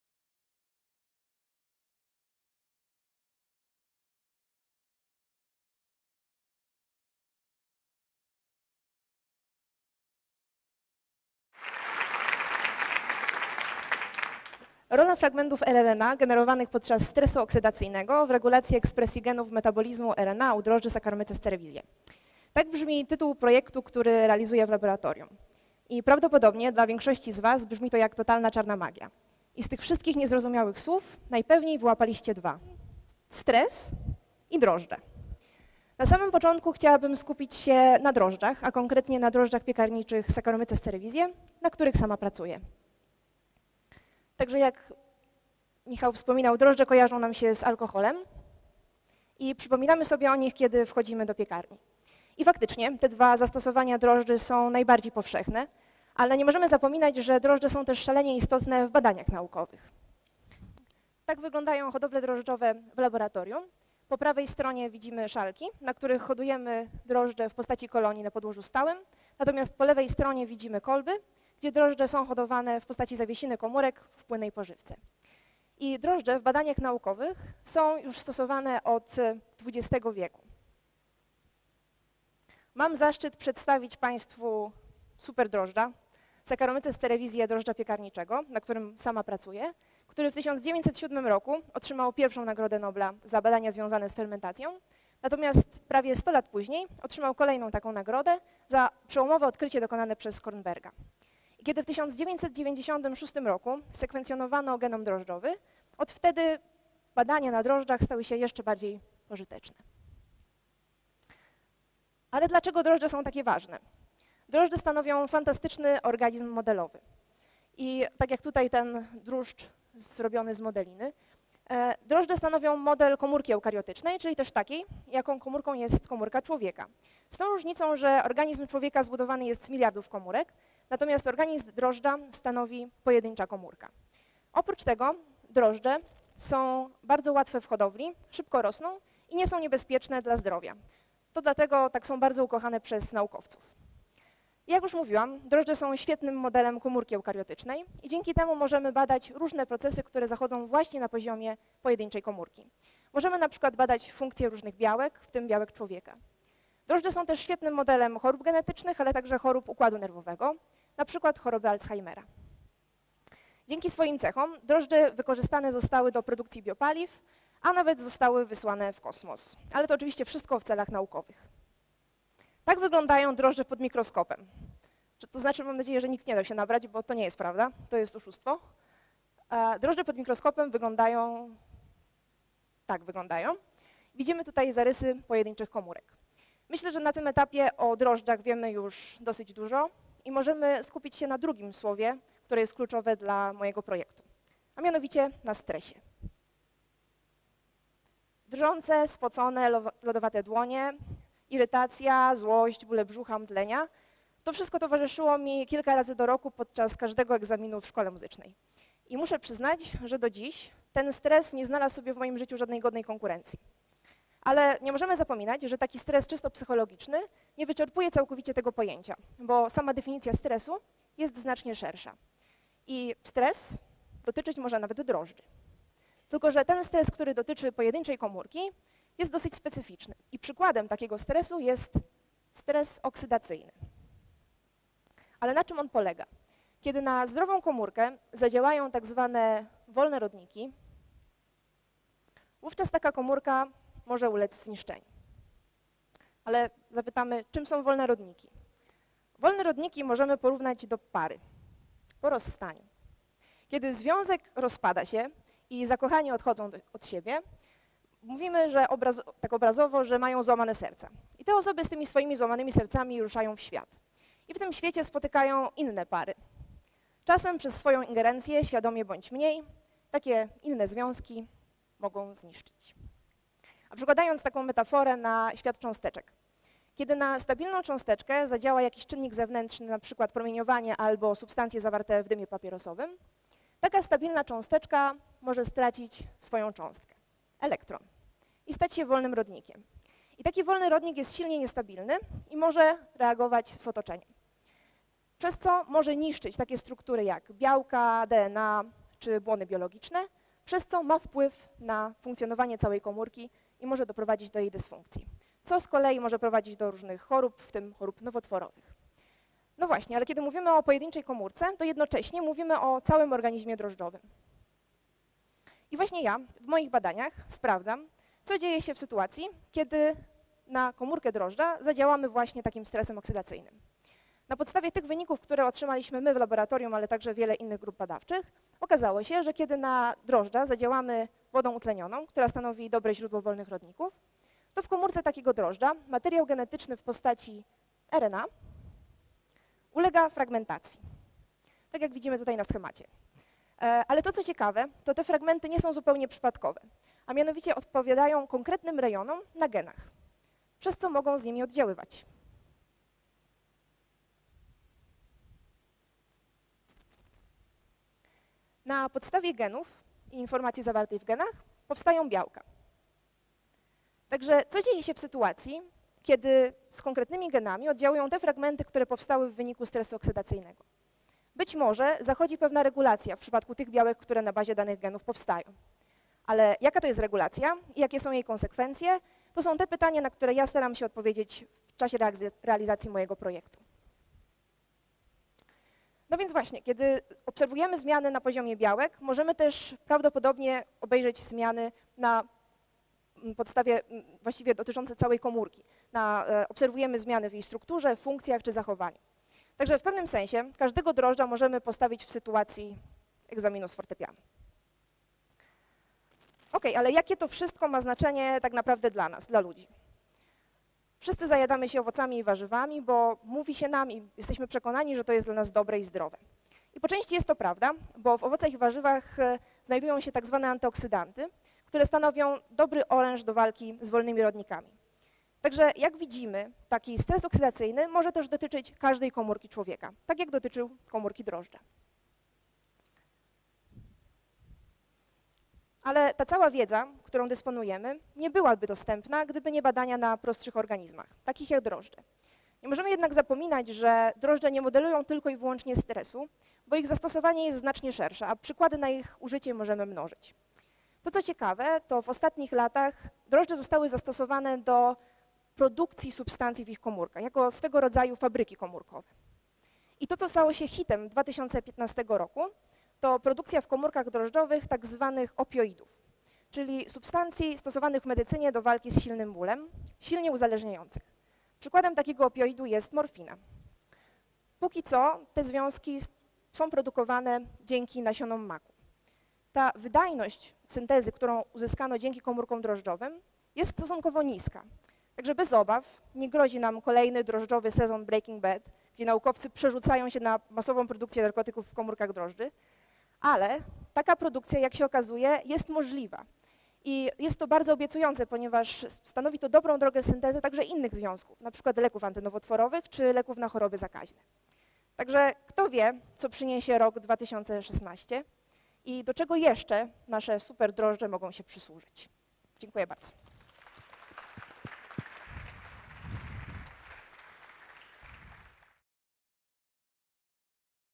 Can yeast be the new Breaking Bad? | Żaneta Matuszek | TEDxUniversityofWarsaw
Have you ever wondered how it feels to be a yeast and be stressed at the same time? Żaneta will tell you something about that! In her speech she reveals the extreme potential of using yeast to facilitate basic research, treat serious illnesses or produce drugs. You will also learn why free-radicals are very similar to a couple after breakup and how to use that knowledge to improve the condition of our health.

Żaneta Matuszek is a molecular biologist. Her scientific focus is RNA particles and their metabolism in eukaryotic cells. She spends most of her time around yeasts, but sometimes she happen to go out and meet people. Whenever she has an opportunity, she contribute to popularisation of science with great joy. She loves music, English sense of humour, and penguins. In her free time, while she is not travelling, she plays the piano, sing, and watch funny videos on the Internet.